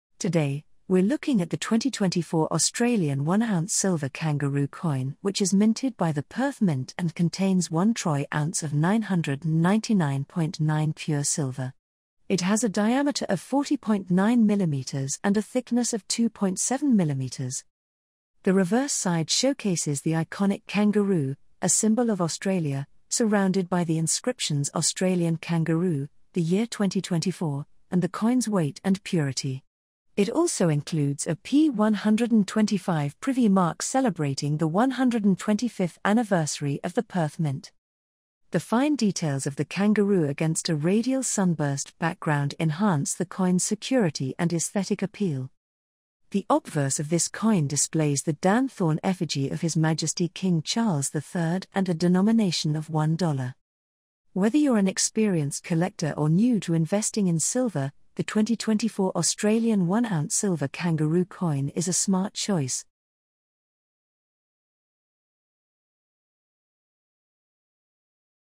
2024 1oz Silver Kangaroo Coin - Essential for Your Collection
The reverse of this new 2024 one-dollar fine silver coin displays a bounding Kangaroo set against a stylized sunburst background.

The obverse of this coin features the Dan Thorne effigy of His Majesty King Charles III.

Key information:
• Manufactured by Perth Mint
• Dimensions (mm): 40.9 x 2.7
• Weight: 31.10
• Fineness: 999.9

🔔 MAKE SURE TO TURN ON YOUR NOTIFICATIONS